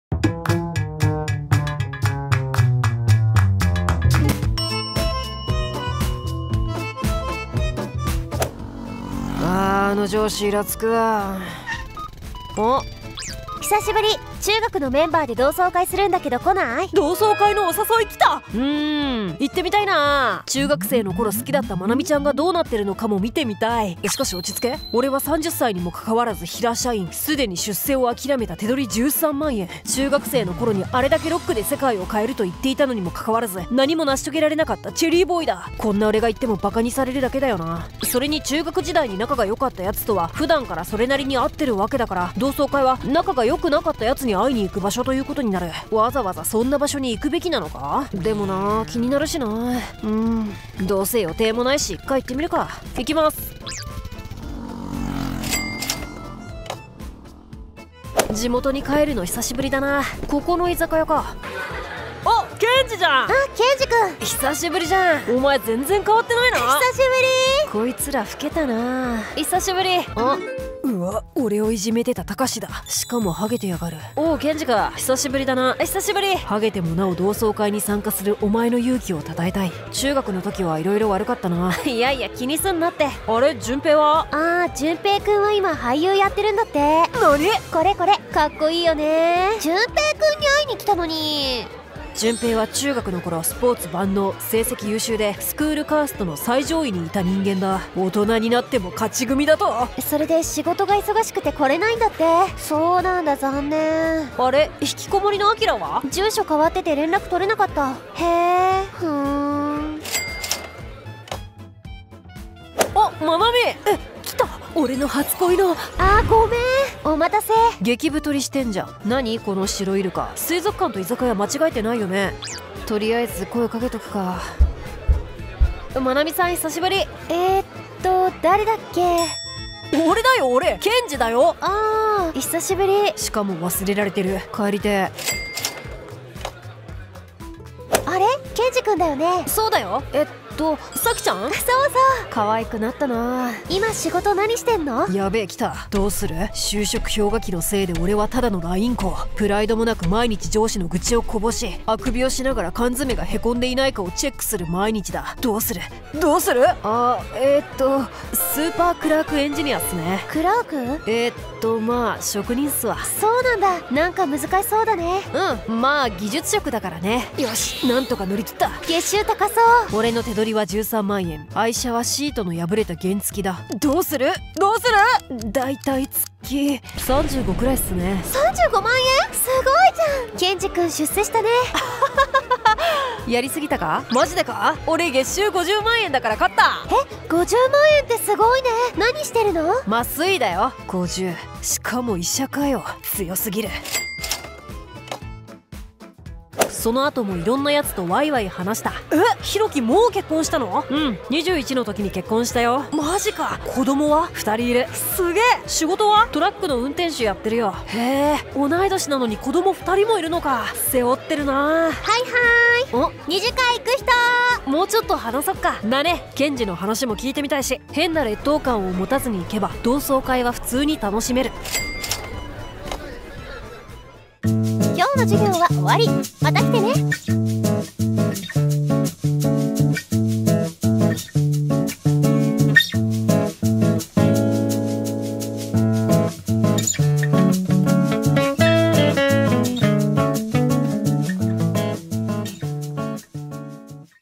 【費米研究所】同學會的格差社會很嚴重【動態漫畫】
第一名「自大」第二名「老去」第三名是什麼？

大家好 這裡是個追求「究竟會怎樣？」的自由研究頻道！是日本人氣漫畫頻道フェルミ研究所的中字官方頻道！
皆さんこんにちは、こちらはフェルミ研究所の中国語公式チャンネルです！